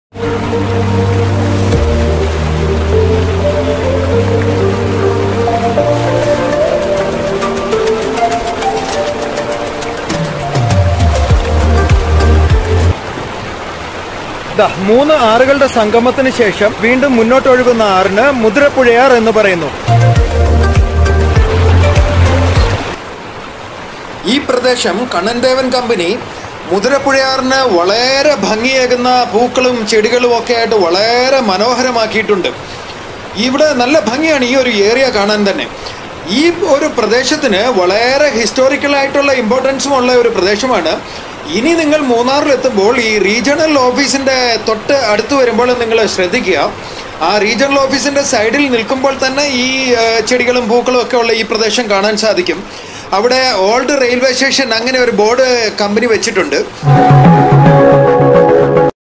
മൂന്ന് ആറുകൾ ഒന്ന് ചേർന്ന് മൂന്നാർ ആയി.munnar kerala Munnar sightseeing
Three rivers confluence and become one river That hence the name Munnar.one river originates Nallathanni estate is Nallathanniyar. Another river kanniyar originates from Kannimala. These two rivers merge near to Munnar Marayur road starting area. Another river Kuttiyar coming from Mattupetty area and confluence these rivers, After that this river known as Muthirappuzhayar. Three rivers confluence that hence the name Munnar.munnar travel kerala tourism idukki Travel with Binu George